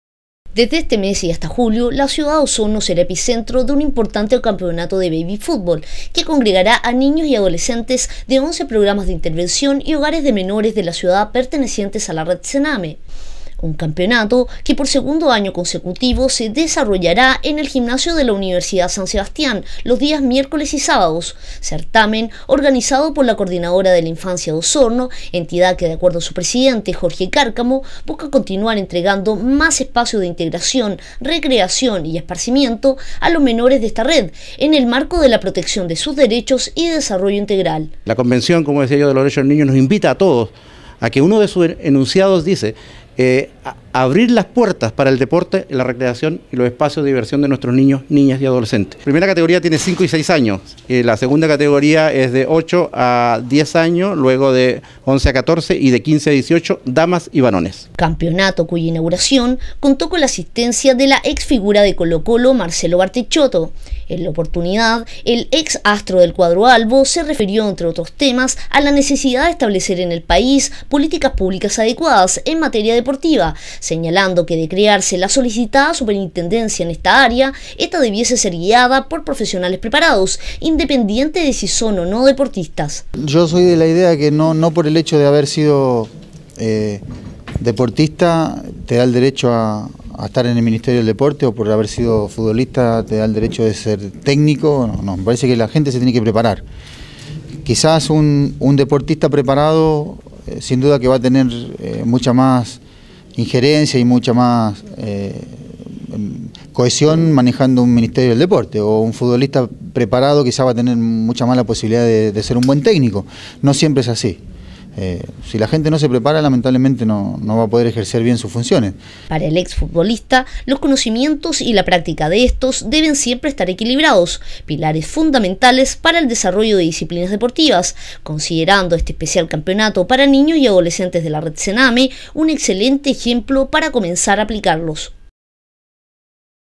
Barticciotto inauguró campeonato en Osorno
Con la asistencia del ex delantero y Director Técnico de Colo Colo, Marcelo Barticciotto, se dio el "vamos" al  campeonato de baby fútbol organizado por la Coordinadora de Infancia de Osorno.